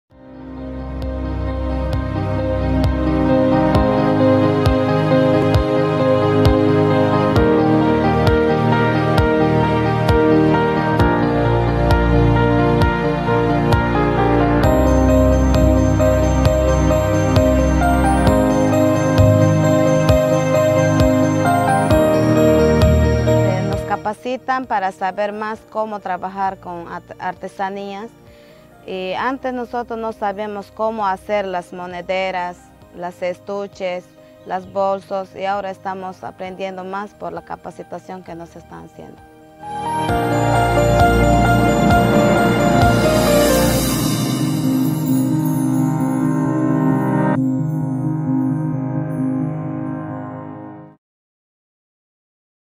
The Forest Alliance: Sustainable development for indigenous communities
The Forest Alliance is an initiative that seeks to improve the quality of life for 350 families of the Shipibo Conibo and Cacataibo Indigenous Peoples in Ucayali, as well as the conservation of Amazon forests and their biodiversity. The Forest Alliance is an initiative of AIDER, USAID and Mirova Natural Capital. The Alliance is a component of our Nii Kaniti project, and through Mirova, we represent the carbon credit generation arm of this initiative.   Climate finance is one of the ways in which project activities are being supported on the ground.

Thank you to AIDER Perú for creating this video. See here for more information on the project

Be sure to check out our blog on the Forest Alliance